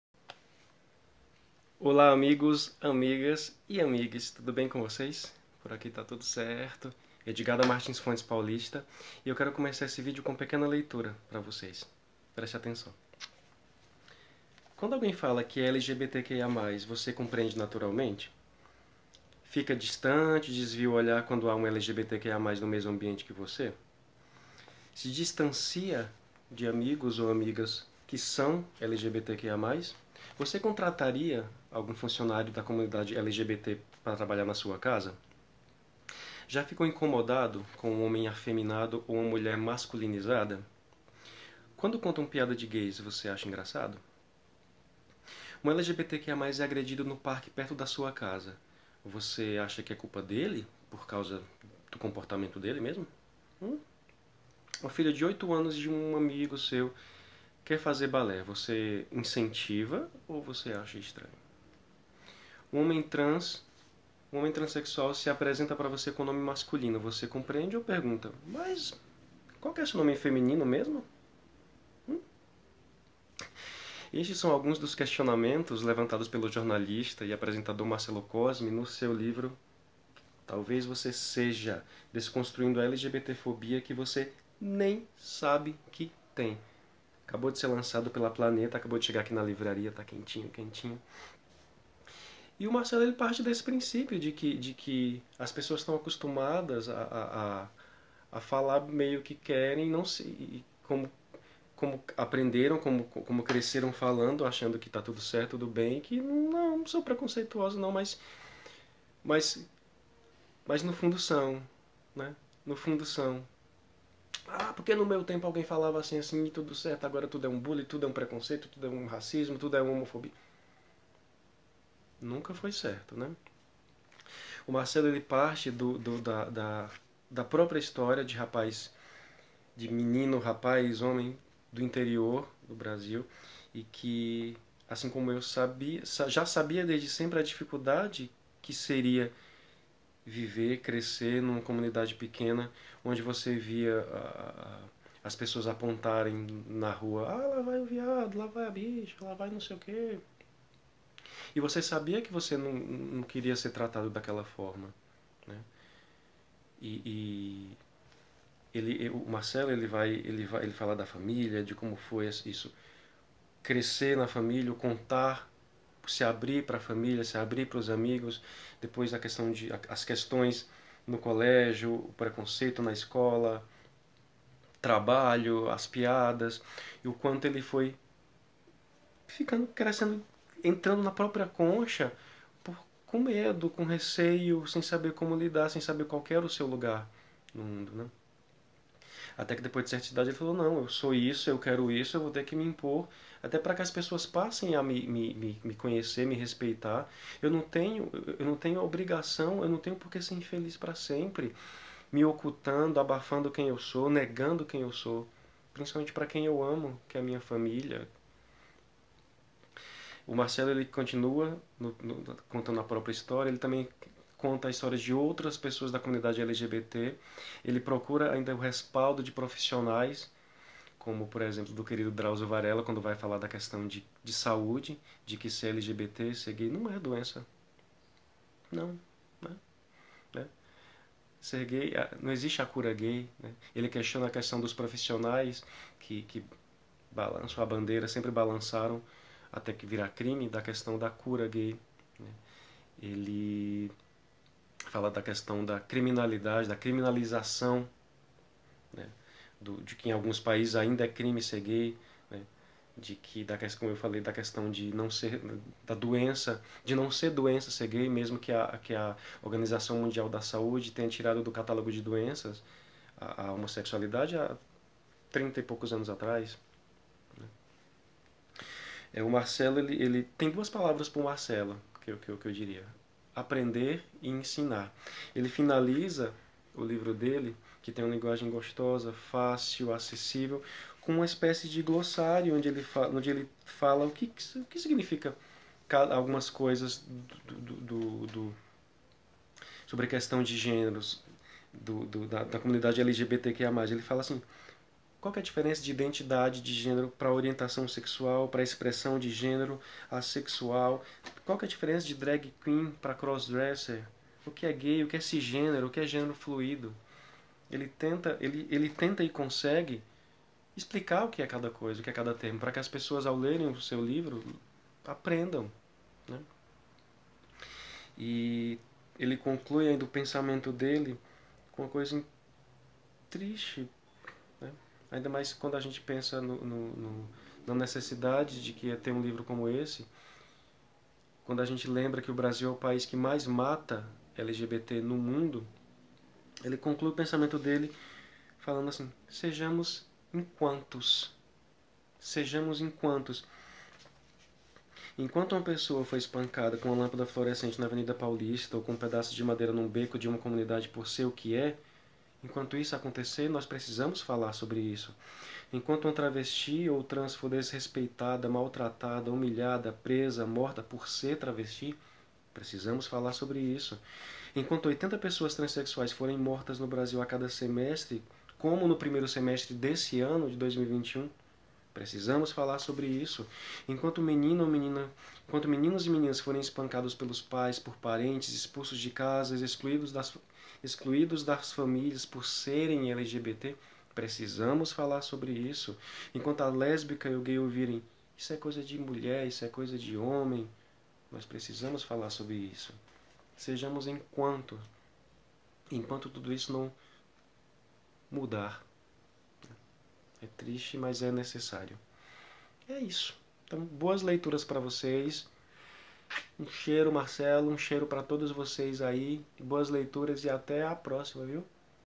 ESTE LIVRO É GAY! - "Talvez você seja"
"Talvez você seja...desconstruindo a lgbtfobia que você nem sabe que tem", do jornalista e apresentador Marcelo Cosme @mcosme, publicado pela @planetadelivrosbrasil é o livro recomendado por @edgarbs_ no vídeo dessa sexta-feira marota! 📚🌈😍

Prestem atenção, é importante!
Boas leituras sempre!